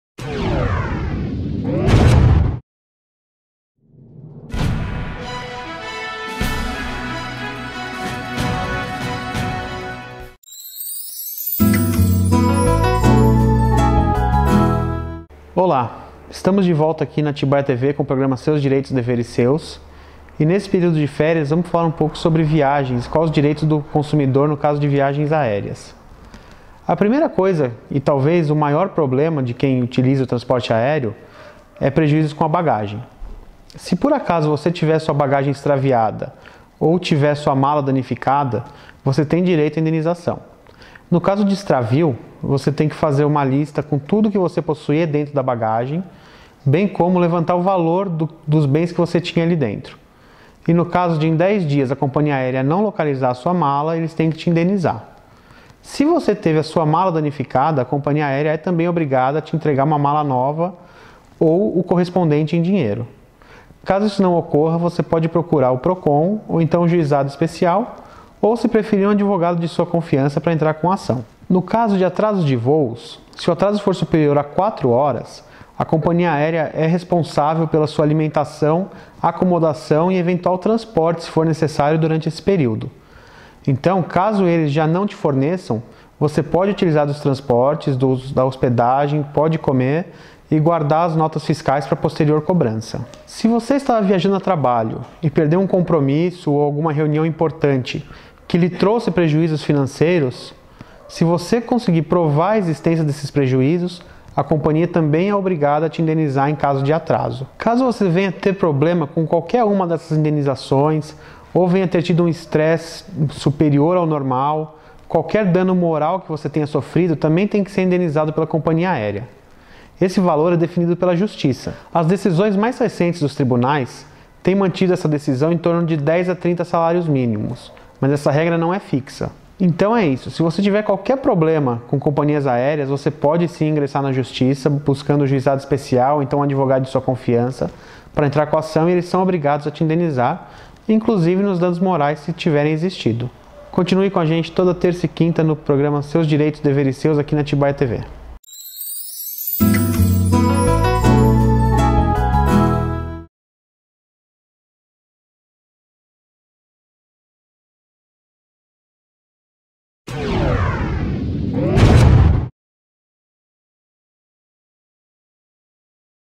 Seus Direitos Deveres Seus #21 - Viagens Aéreas
Saiba quais são os direitos do consumidor em viagens aéreas.